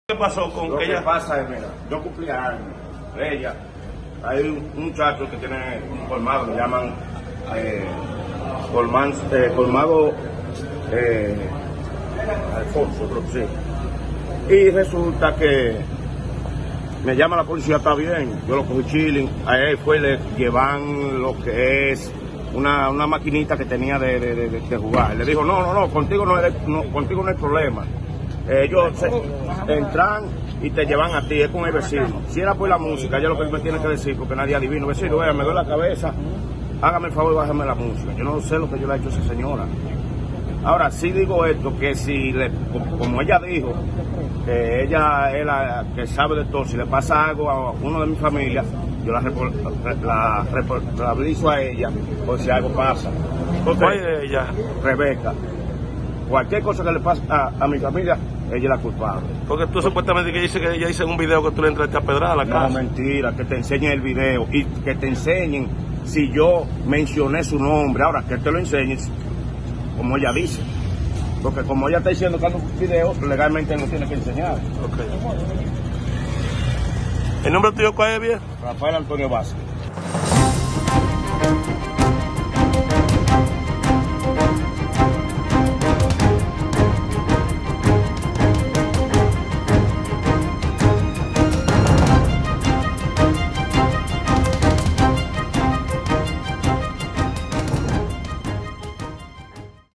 Detienen hombre acusado de apedrear vivienda de coordinadora regional de Derechos Humanos
SAN FRANCISCO DE MACORÍS.- Agentes de la Policía Nacional apresaron a un hombre acusado de apedrear la vivienda de la coordinadora regional nordeste de la Comisión Nacional de los Derechos Humanos.
El detenido fue identificado como Rafael Antonio Vasquez  quien negó haber apedreado la vivienda de Rebeca Henriquez.

Vasquez responsabilizó a Henríquez de cualquier cosa que le suceda tanto a él como a su familia.